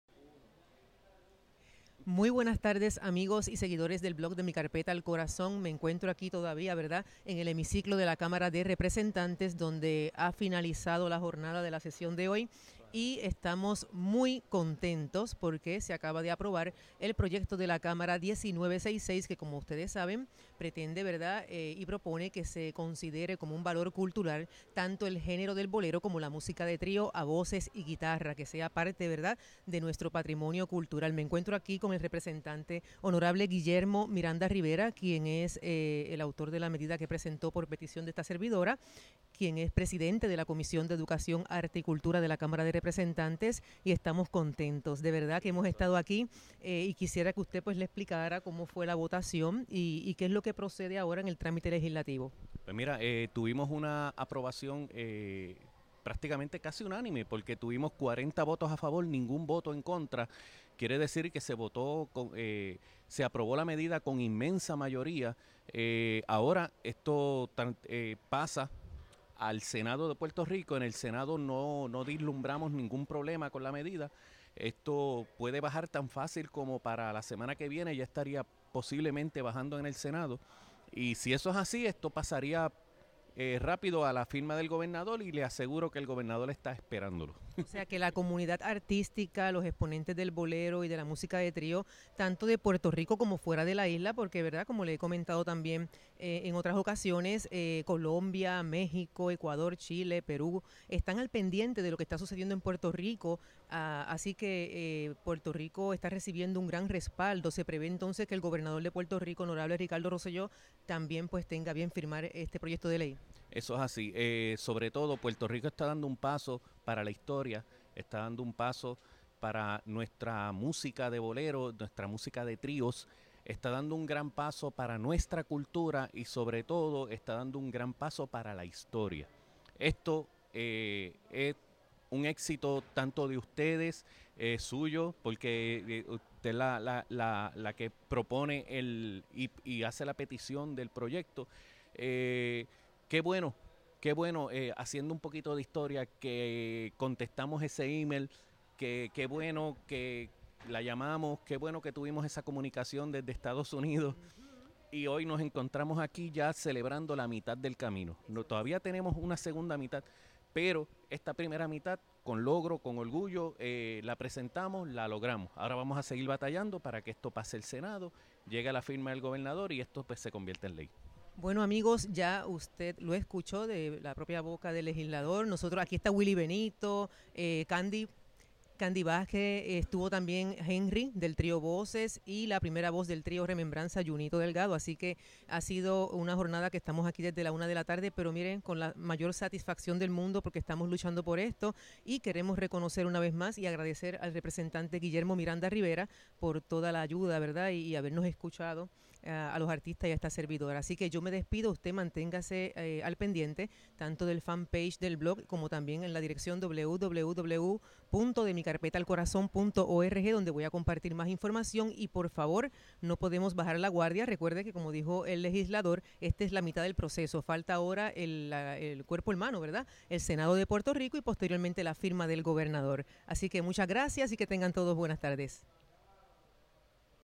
Entrevista al representante Miranda Rivera, ante la aprobación del P. de la C 1966
Rumbo al Senado de PR, Proyecto que pretende declarar el género del bolero y la música de trío, a voces y guitarras, como patrimonio cultural de Puerto Rico.